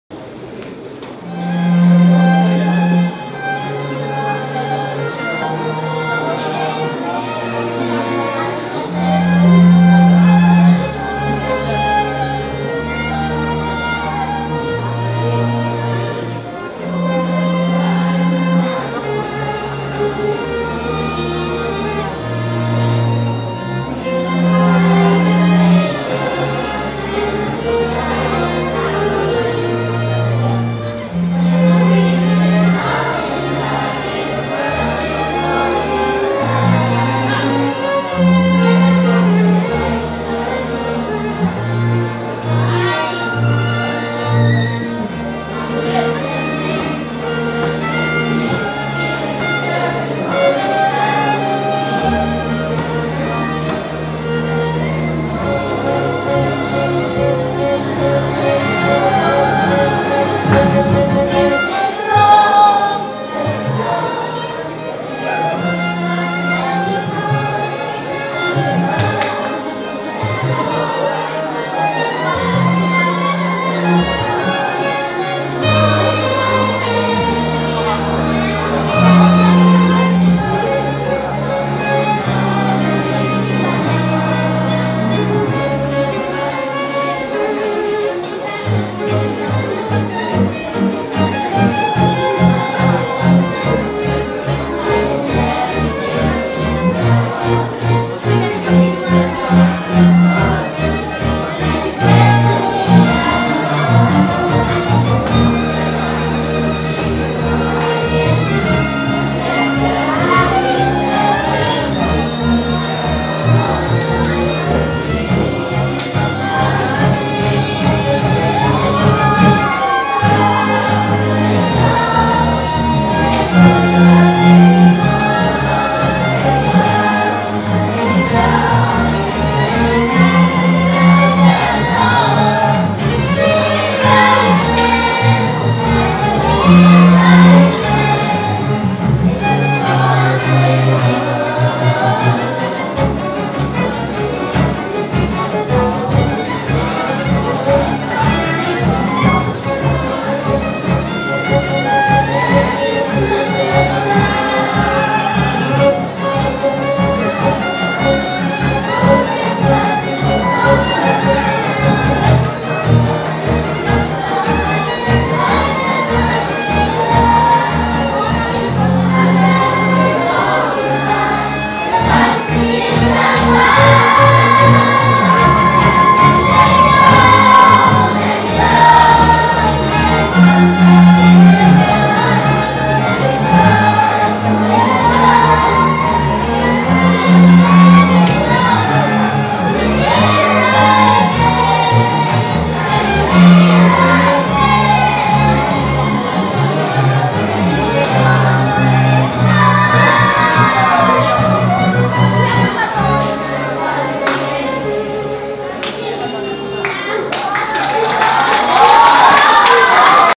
Manila String Machine at SOSManilaAt30 - Part 1
Manila String Machine's performance during SOS Children's Village in Manila's 30th Anniversary.
today and make a difference in the lives of orphaned, abandoned and neglected Filipino Children.